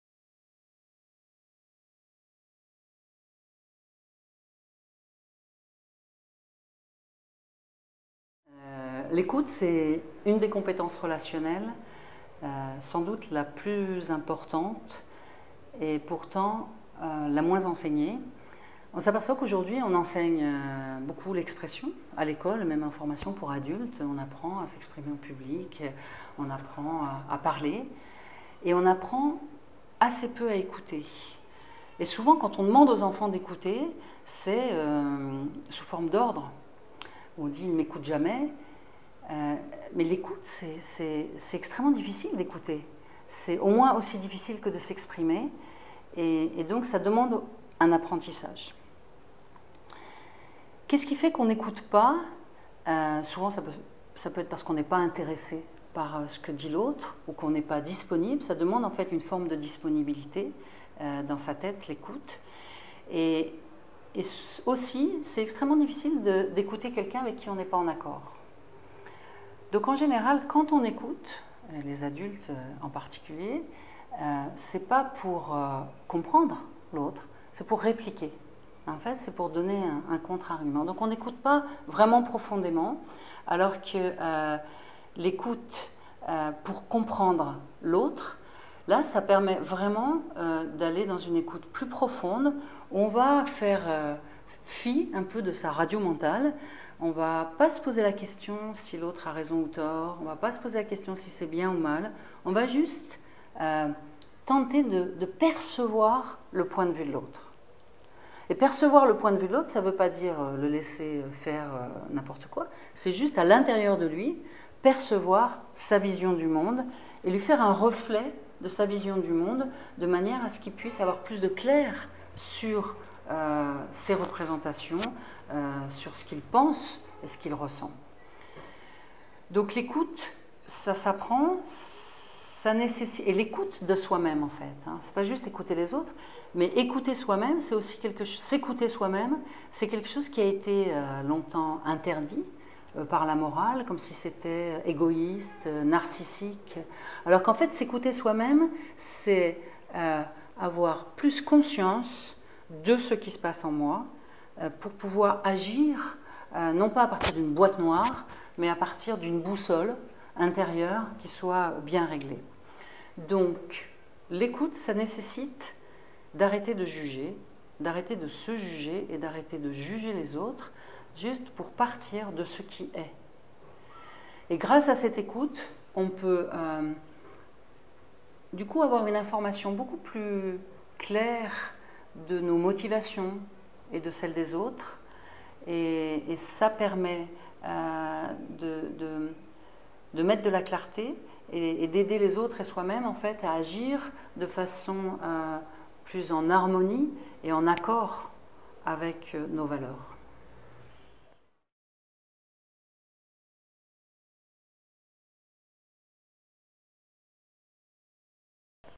Etincelle 12 - De l’importance de l ecoute
Interview de Véronique Guérin, directrice de l'association Etincelle, organisme de formation en développement relationnel.